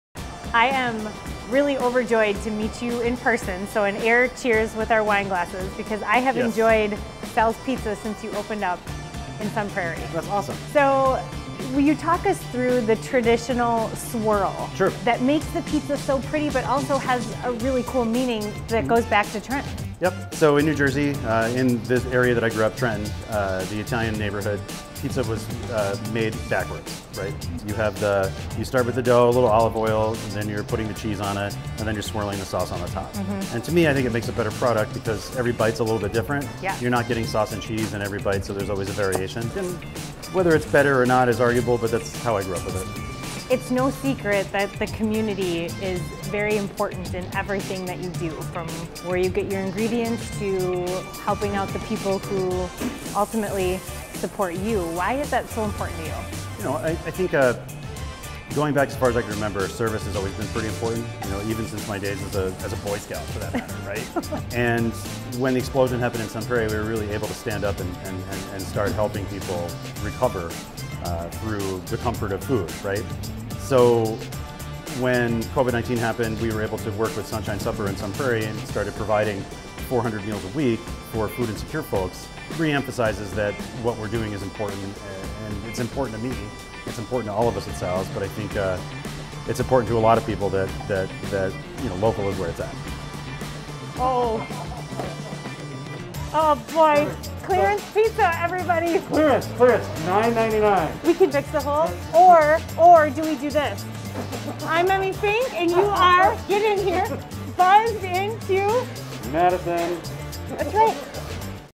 Salvatore's Tomato Pies
Madison has a ton of great pizza, but this is one of the best.  We had the Thai chicken pizza - out of this world!